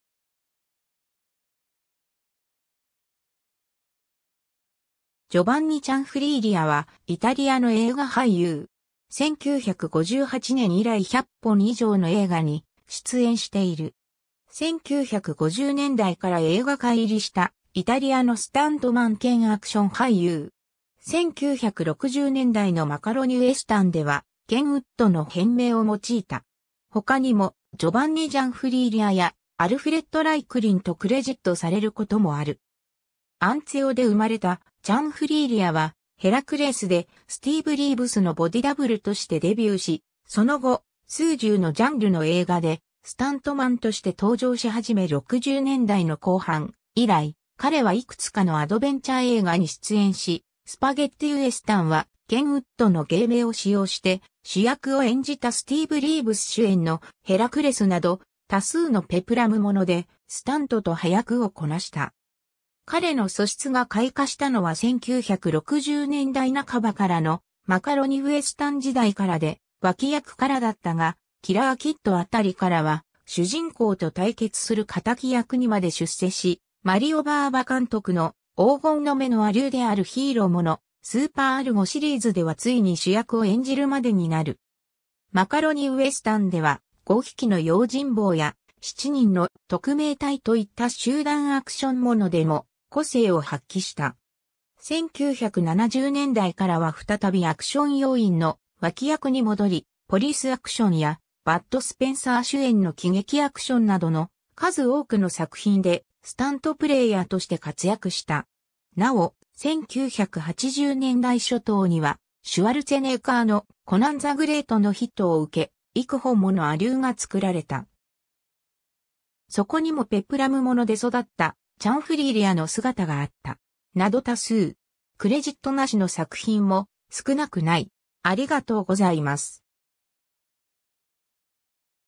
ジョヴァンニ・チャンフリーリア
ジョヴァンニ・チャンフリーリア（Giovanni Cianfriglia、1935年4月5日 - ）はイタリアの映画俳優。
 1958年以来100本以上の映画に出演している。
1950年代から映画界入りしたイタリアのスタントマン兼アクション俳優。
1960年代のマカロニ・ウエスタンではケン・ウッドの変名を用いた。
他にもジョヴァンニ・ジャンフリーリアやアルフレッド・ライクリンとクレジットされることもある。
 アンツィオで生まれたチャンフリーリアは、ヘラクレスでスティーブリーブスのボディダブルとしてデビューし、その後、数十のジャンルの映画でスタントマンとして登場し始め60年代の後半以来、彼はいくつかのアドベンチャー映画に出演し、スパゲッティウエスタンは、ケンウッドの芸名を使用して主役を演じた スティーヴ・リーヴス主演の『ヘラクレス』(1957)など多数のペプラム物でスタントと端役をこなした。
彼の素質が開花したのは1960年代半ばからのマカロニ・ウエスタン時代からで、脇役からだったが、『キラー・キッド』(1967/未/テレビ放映)辺りからは主人公と対決する敵役にまで出世し、マリオ・バーヴァ監督の『黄金の眼』(1968)の亜流であるヒーロー物『スーパーアルゴ』シリーズでは遂に主役を演じるまでになる。
 マカロニウエスタンでは『五匹の用心棒』(1966)や『七人の特命隊』(1968)といった集団アクション物でも個性を発揮した。
1970年代からは再びアクション要員の脇役に戻り、ポリス・アクションやバッド・スペンサー主演の喜劇アクションなどの数多くの作品でスタント・プレイヤーとして活躍した。
尚、1980年代初頭にはシュワルツェネッガーの『コナン・ザ・グレート』(1982)のヒットを受け、幾本もの亜流が作られた。
そこにもペプラム物で育ったチャンフリーリアの姿があった。
 など多数。
クレジット無しの作品も少なくない。